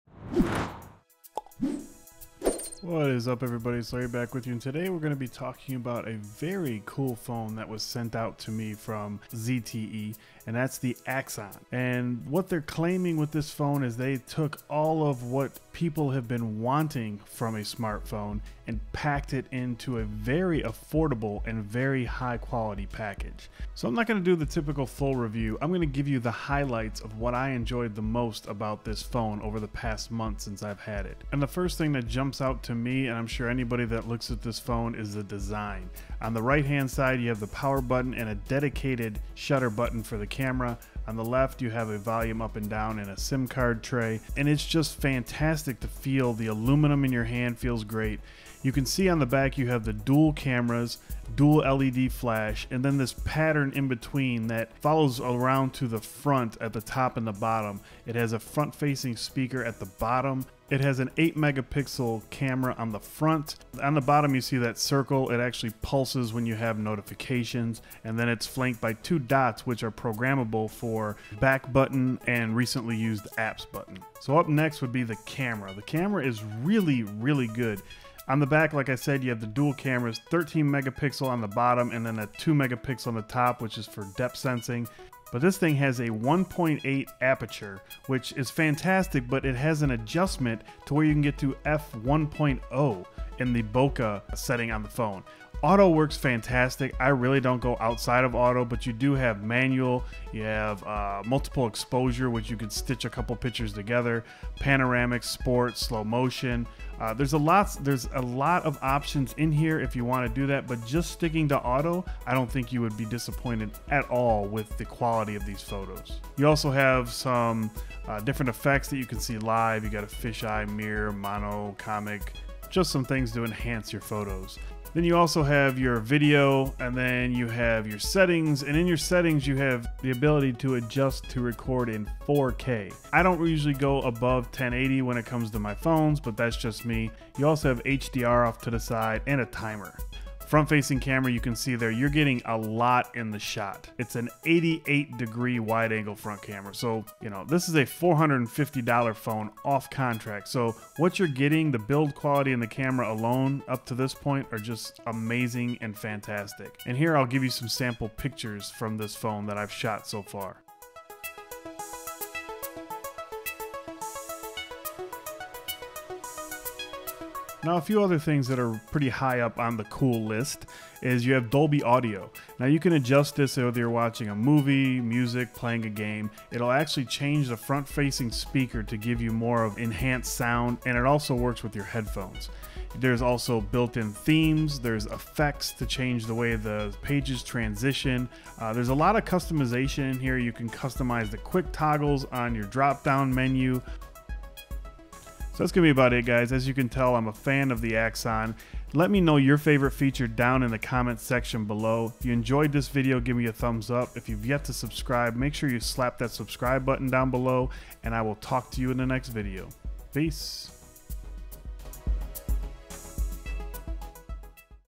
Top Features on the Axon phone by ZTE
Watch for a written review on geekynerddad.com coming soon, we will go in depth on the features mentioned in this video as well as other hits and misses of the Axon by ZTE.

When ZTE sent me this phone for review I wasn't sure what to expect but I was pleasantly surprised!

** Phone was Provided by ZTE

Equipment and Gadgets I use!